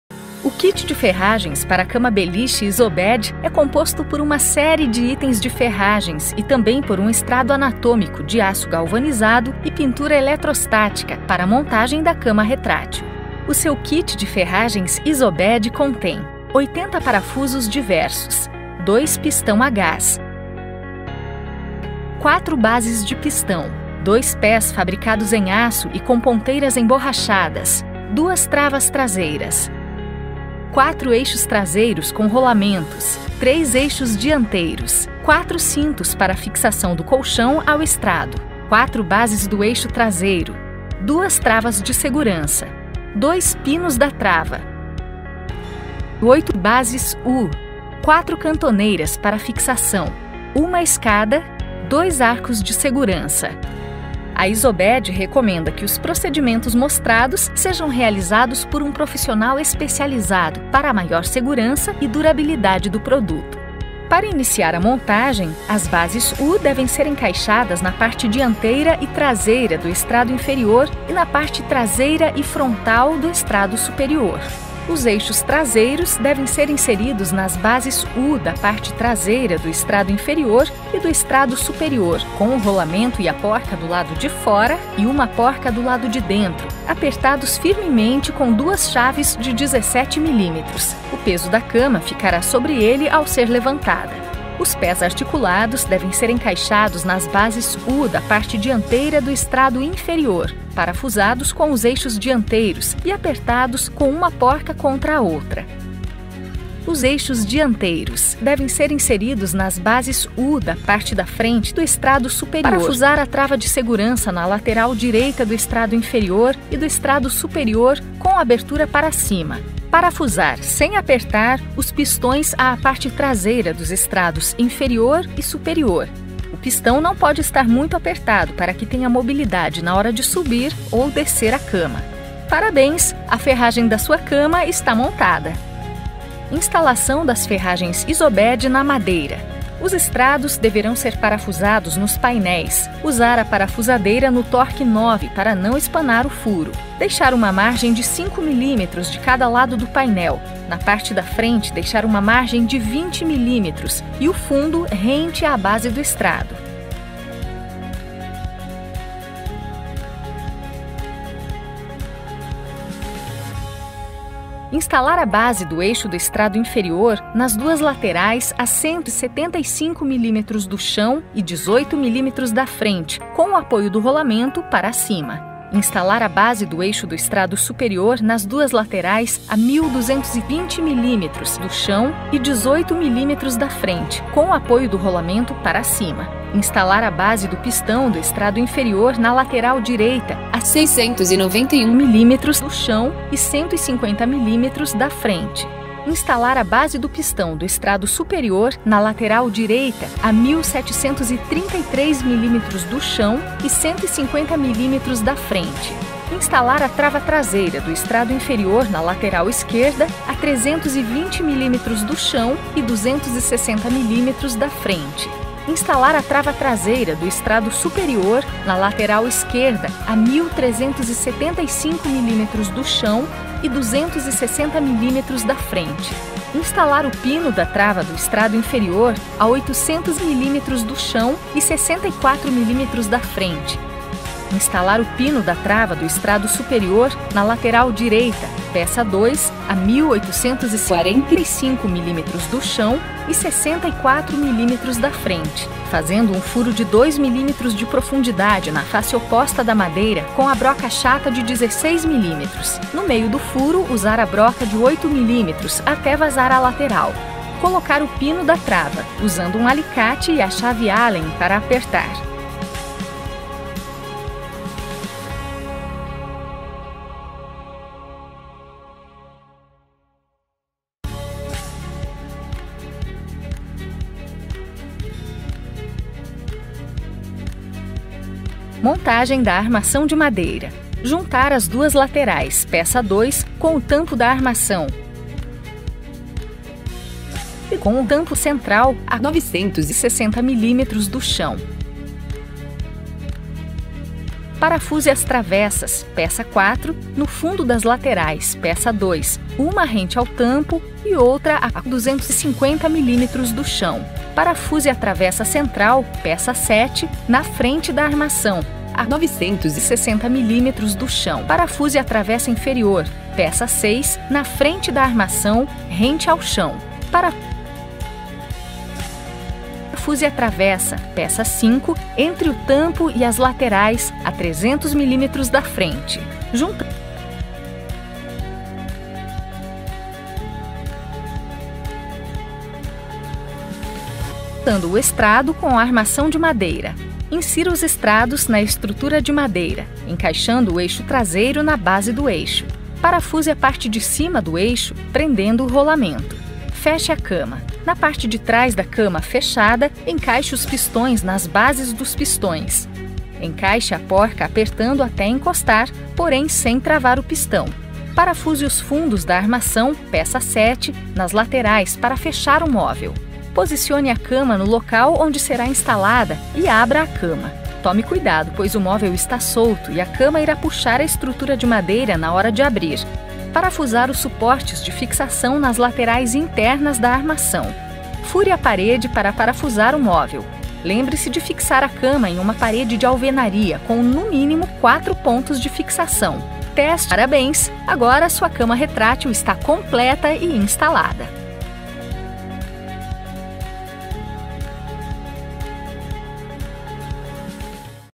Cama Retrátil & CIA - Vídeo 3D montagem - Cama Beliche
Vídeo em 3D do passo a passo completo da Cama Retrátil & CIA

Trazendo mobilidade e aumentando a área útil do seu Studio ou Apartamento, as ferragens da camaretratilecia.com são devidamente testadas e certificadas, além de serem produzidas com materiais de primeira linha.

Cama retrátil completa de embutir na parede com estrado anatômico em aço galvanizado e ripas flexíveis de extrema qualidade e design.